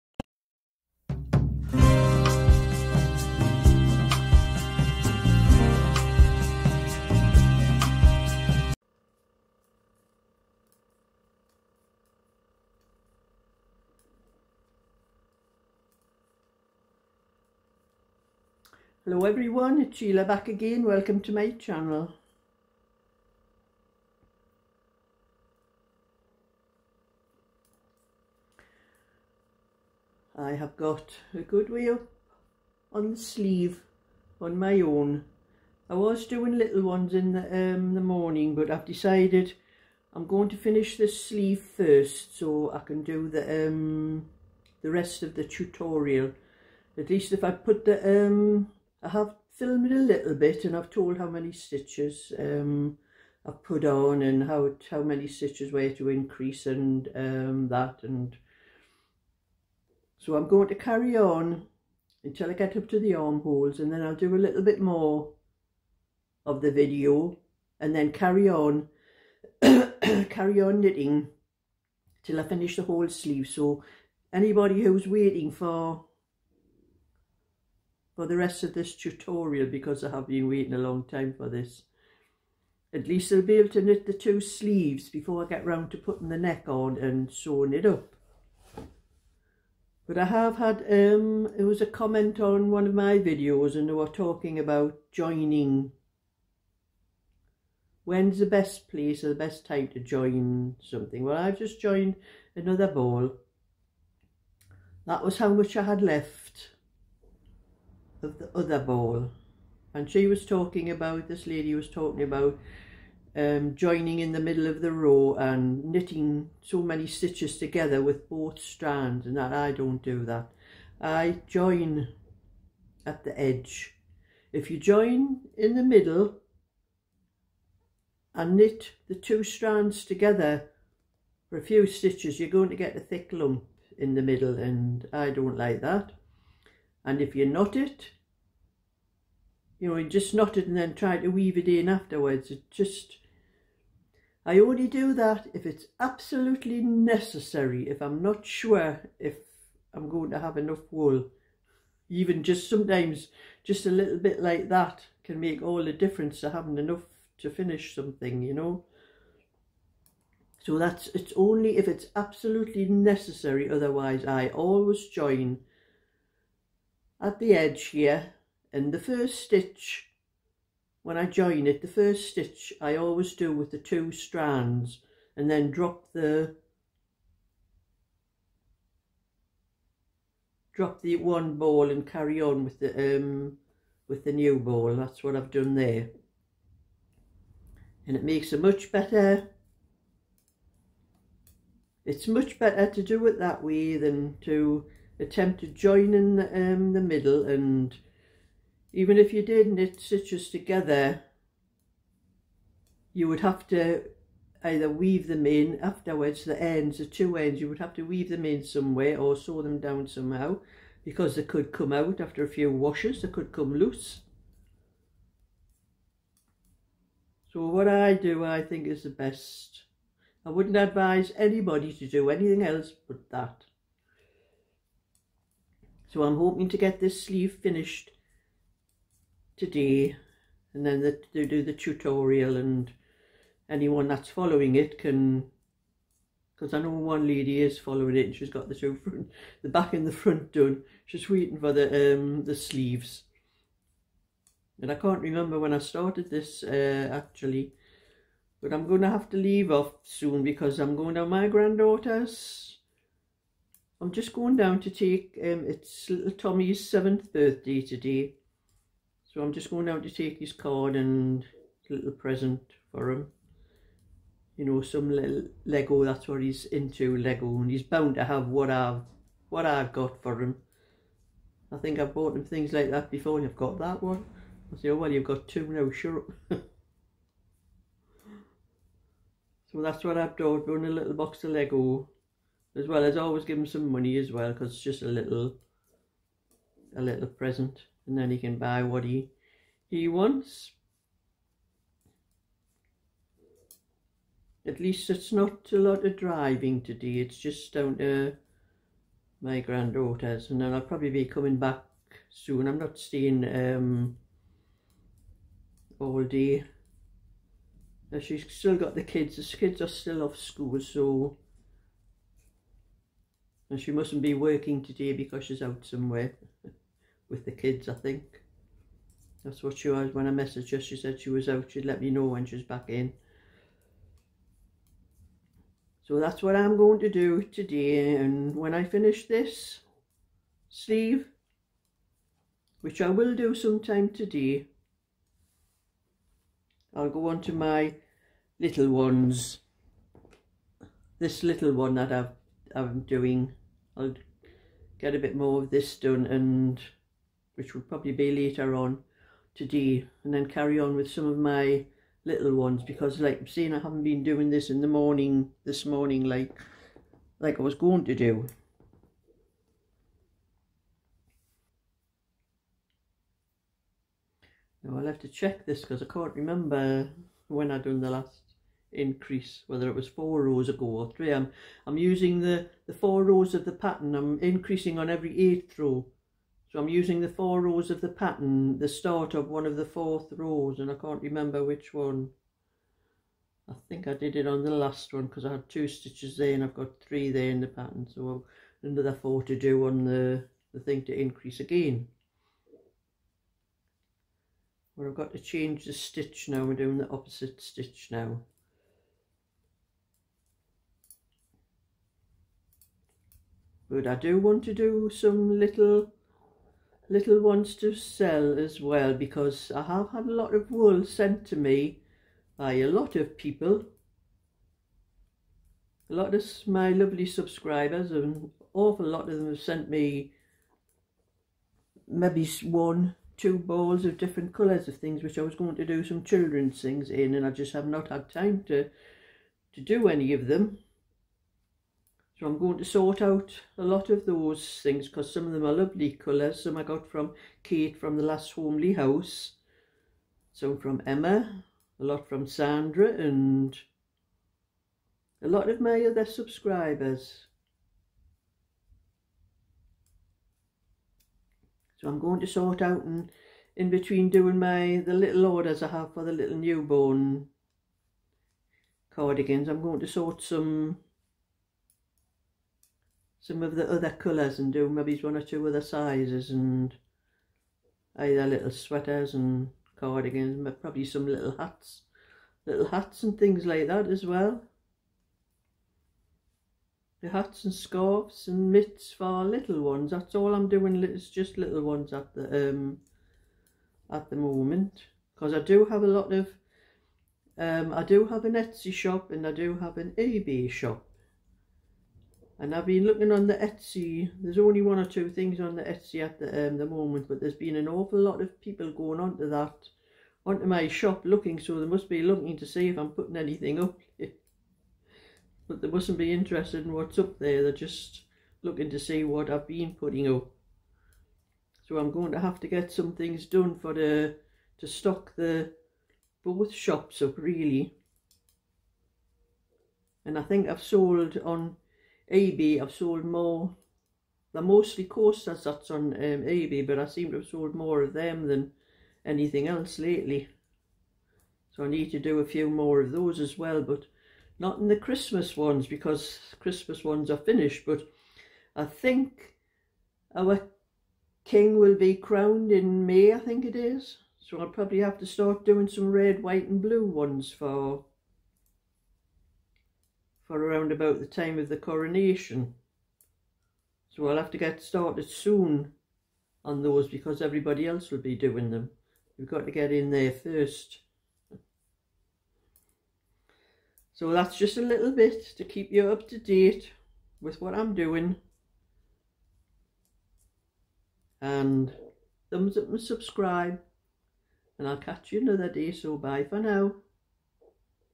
Knitting The 42inch Lemon Sweater to Finish My Tutorial, Sheila's Knitting Tips and Other Stuff
i must get a move on with the lemon sweater people waiting for the rest of the tutorial..